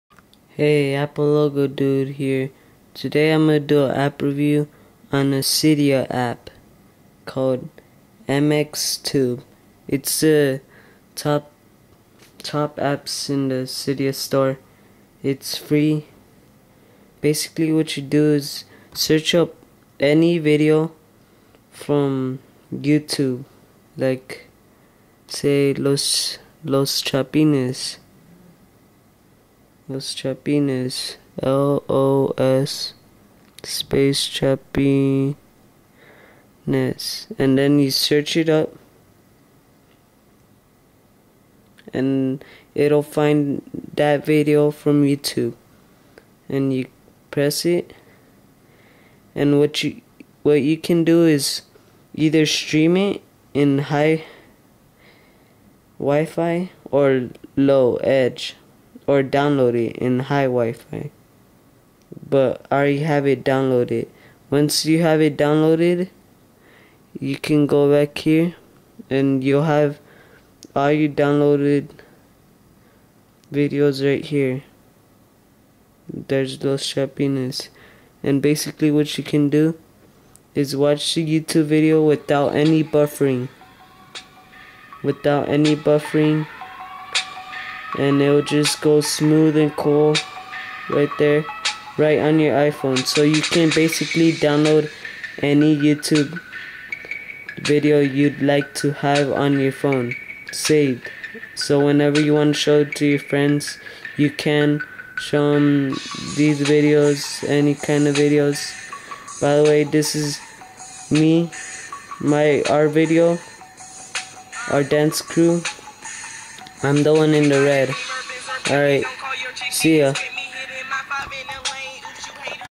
Youtube Downloader: Cydia App Review
MxTube Cydia App:
With this app your able to download any youtube video you'd like, that way when on the road you'll be able to show your family, friends some youtube videos you'd like to show without any buffering.

Give me a THUMBS UP for the great tutorial.